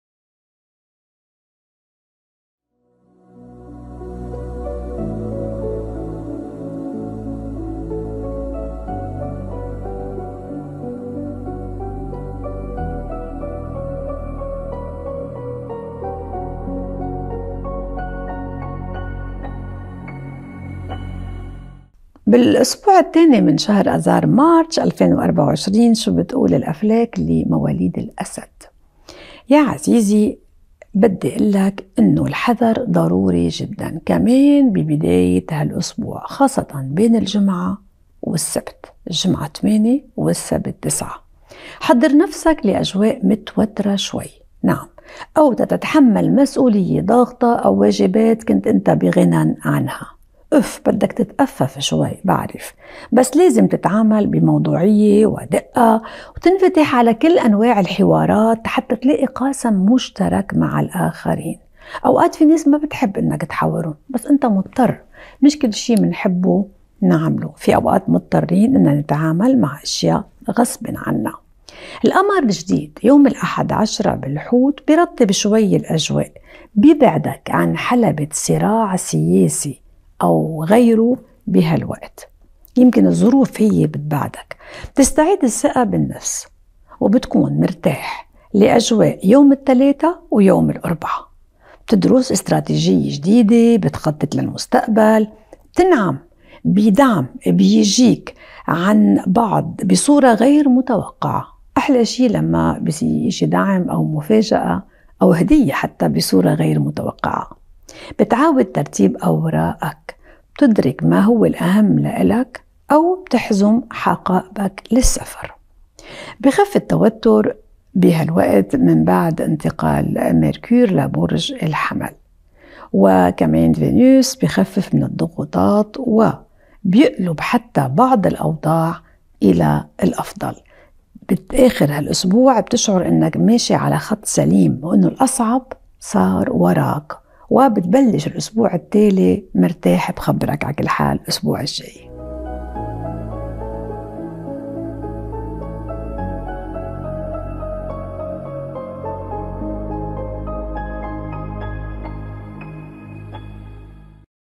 ماغي فرح - برج الأسد - الأسبوع 2 من شهر آذار - مارس - ٢٠٢٤/ Maguy Farah - Leo - week 2- March 2024
ماغي فرح - برج الأسد - الأسبوع الثاني من شهر آذار - مارس - ٢٠٢٤/ Maguy Farah - Leo - week 2- March 2024

23 تموز/ يوليو/ July - 22 آب/ أغسطس/ August

مواليد الدائرة الأولى (من 23 تموز/ يوليو/ July إلى 3 آب/ أغسطس/ August)
مواليد الدائرة الثانية (من 4 إلى 13 آب/ أغسطس/ August)
مواليد الدائرة الثالثة (من 14 إلى 23 آب/ أغسطس/ August)

شاهد التوقعات الكاملة لـ الأسد ٢٠٢٤ - Watch the Yearly Leo Horoscope for 2024